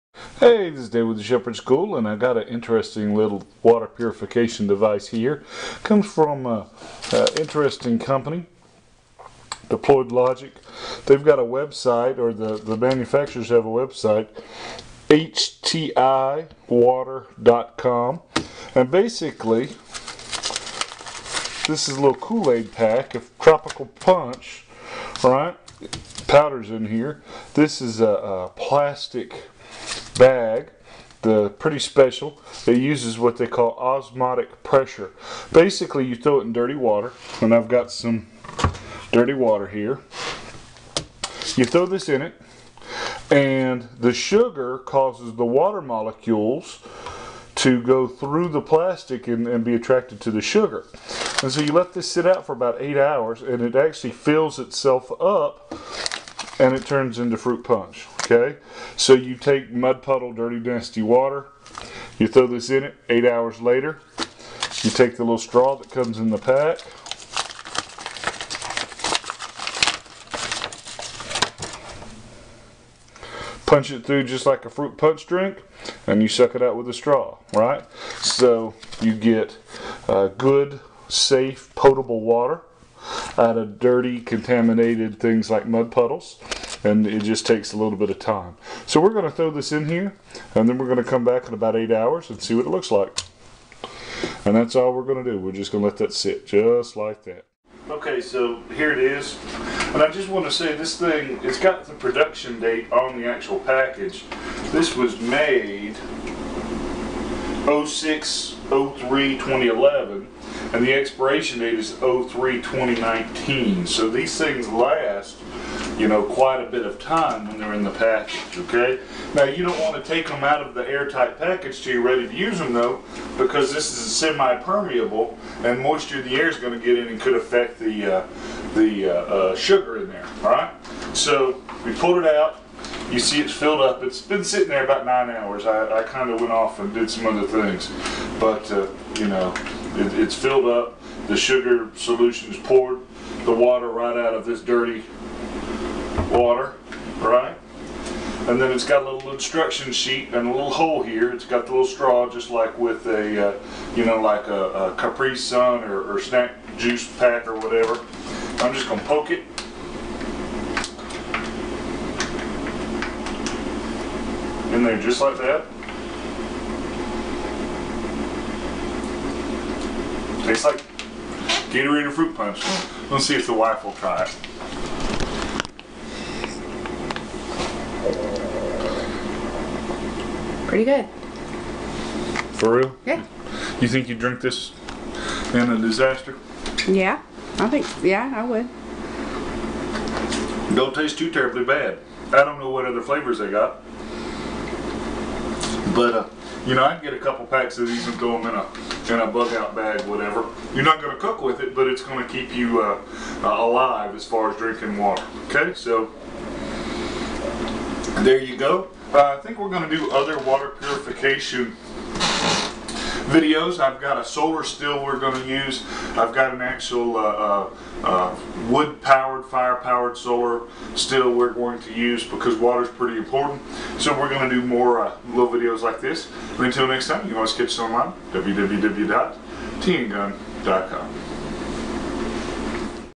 HydroPack Water Purifier Review
While at the National Prepper and Survialist Expo I saw a really cool product, and the vendor (Deployed Logix) was nice enough to give me this product to review and test out.  I have read about this technology before, but I have not had the opportunity to review it before.

According to the manufacturer:

The HydroPack was designed for its ease of use, simply put the filter into virtually any dirty water and the filter goes to work making a clean drink.

The self hydrating pouch produces 12-17 fl. Oz. of sports drink.

The filter uses a passive osmotic process so it never clogs, requires no pumping and filters down to the atomic level (3-5 angstroms -- which is .3-.5 billionths of a meter) so the filter removes even the smallest viruses to the same purity of a complicated reverse osmosis machine.

    Forward osmosis filter requires no pumping, works in any virtually and water, even muddy water. Produces a sports drink for improved hydration.
    Meets or surpasses reductions for bacteria, viruses and cysts as specified by the EPA for water purifiers
    Easiest of our filters to use, simply put the HydroPack into the dirty water and it starts producing a clean drink.
    The highest purity level of any personal filter, non-clogging filter produces a sports drink for improved hydration.

I simply dumped the pack in a bowl of dirty water and went to bed, when I got up, I just stabbed the included straw into the inflated package and drank the contents.
It tasted like a sports drink, and I did not have any negative views on the taste -- my wife even drank some.
I would probably keep some alcohol wipes in my kit to wipe off the location your going to insert the straw to reduce cross contamination, but other than that, I think this has a place in a bug out bag.  I would not keep these in bulk for bugging in due to cost, but I think they are very useful for on the go kits.